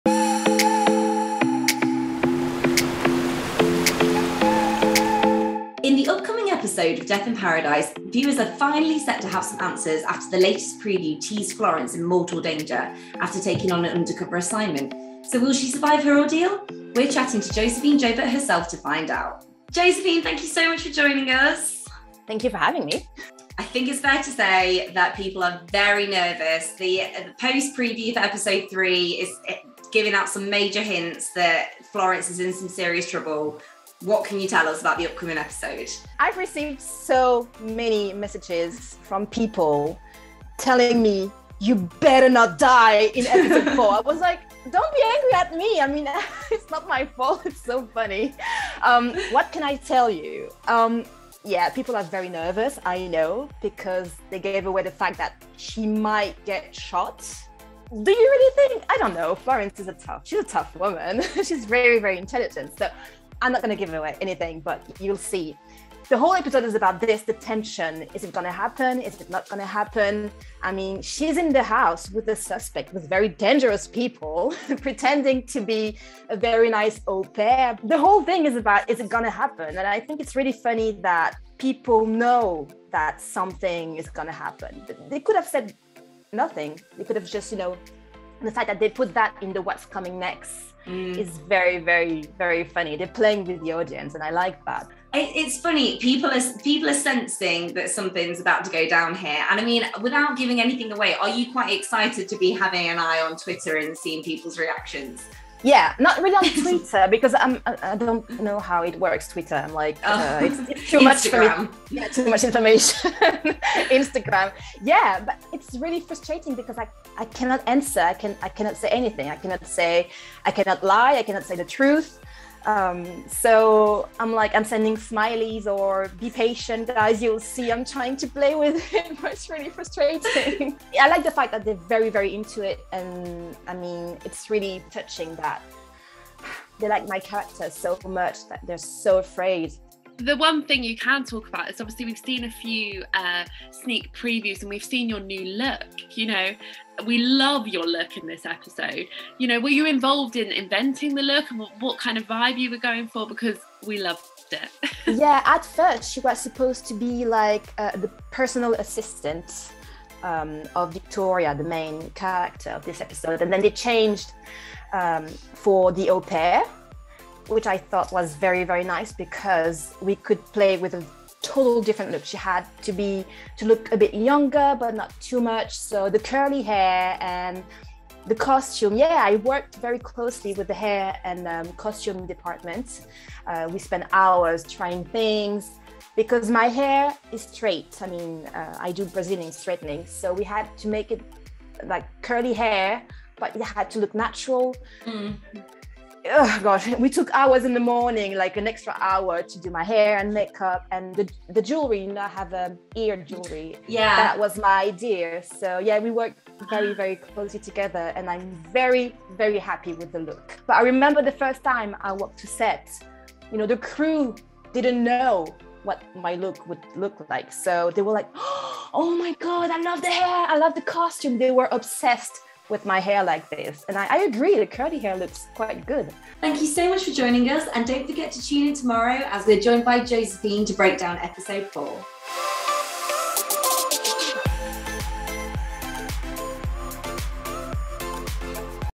Death in Paradise: Josephine Jobert REVEALS if Florence will survive!
Josephine Jobert has joined up ahead of 11x04 to talk all things DS Florence Cassell, who is on an undercover assignment. So will the fan favourite Death in Paradise character make it through the episode? Find out what Josephine had to say!